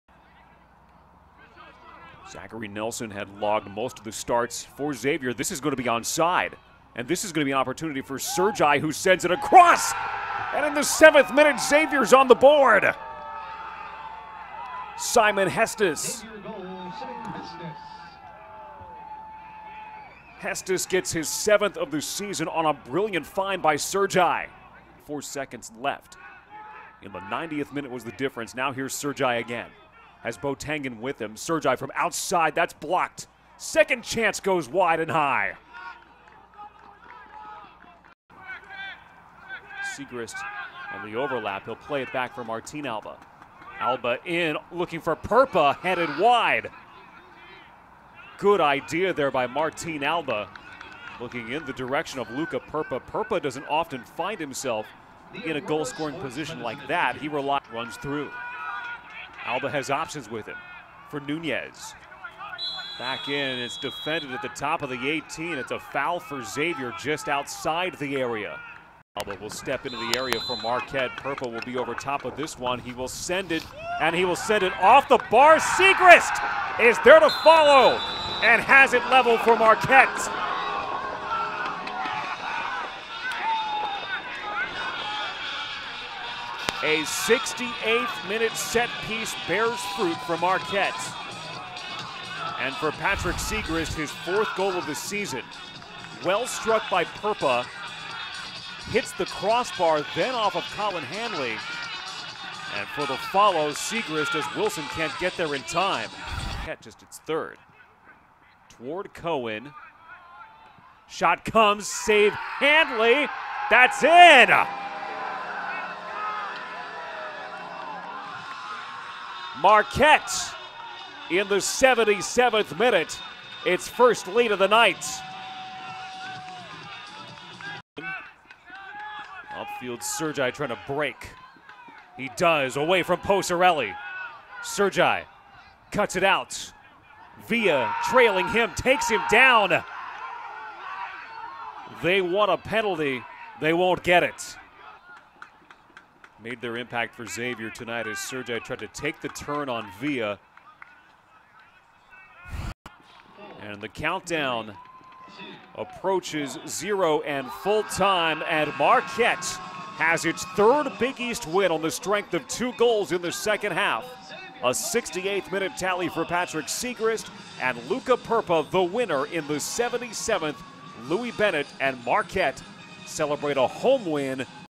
MSOC Match Highlights: Xavier at Marquette
Marquette goals from Patrick Seagrist (68') and Luka Prpa (77') gave the Golden Eagles a 2-1 BIG EAST win over Xavier at Valley Fields on Oct. 20, 2018.  Siman Hestnes (7') scored for Xavier.